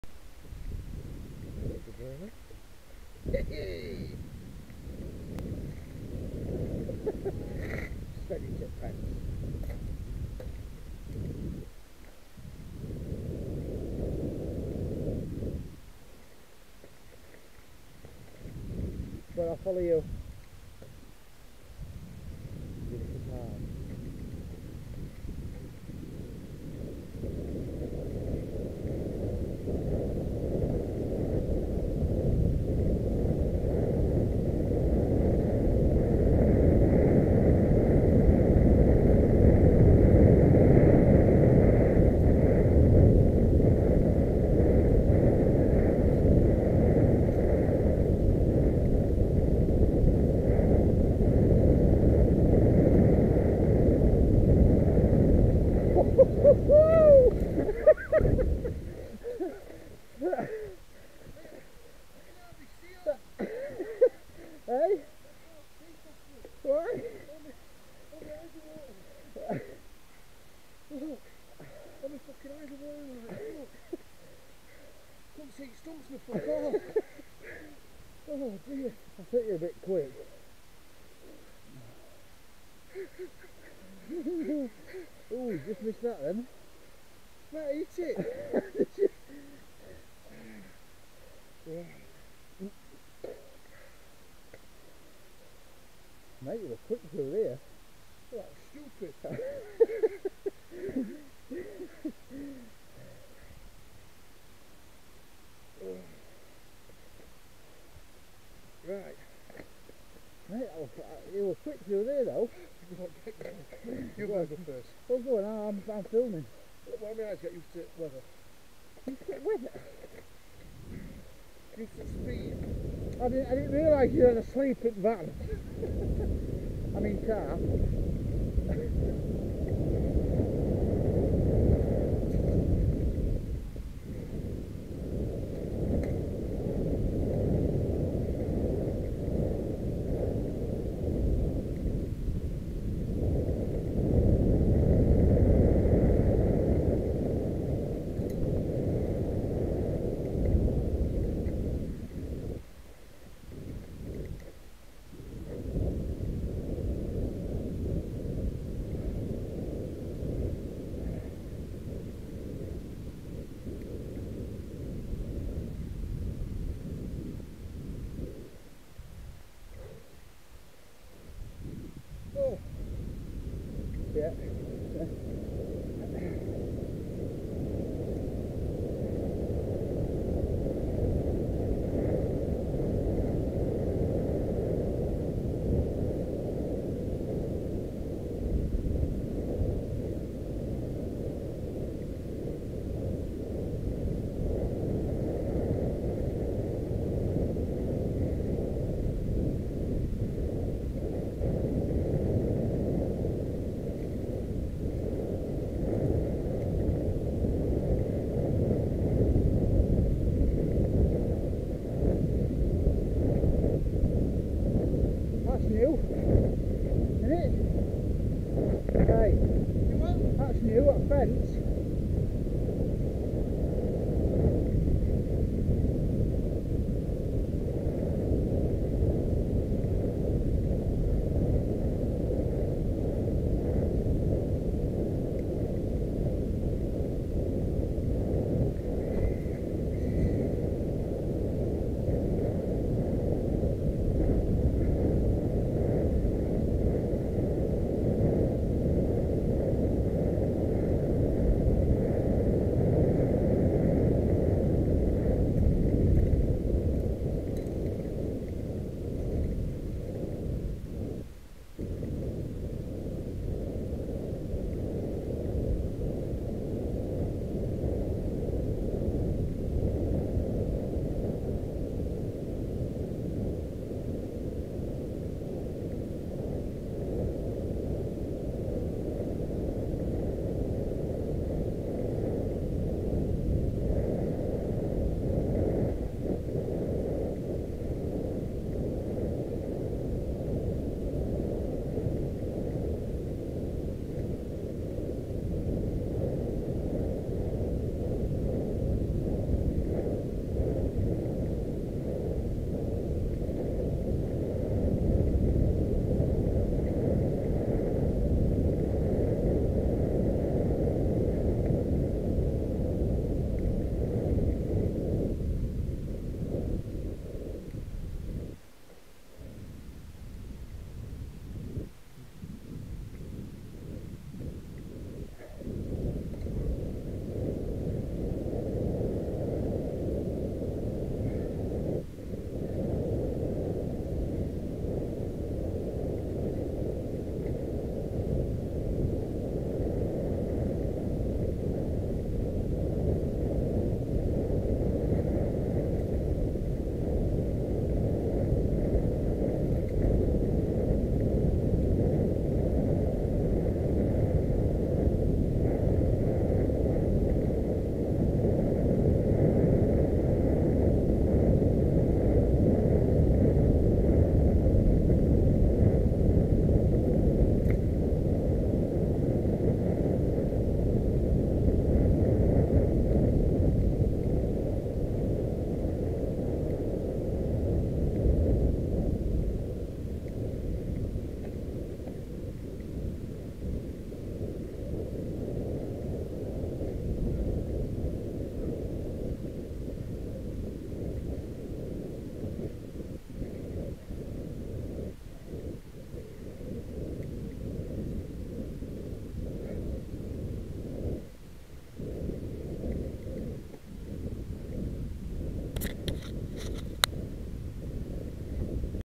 Sherwood Pines 1st crash of the day.MOV
Sherwood Pines first section and first fall of the day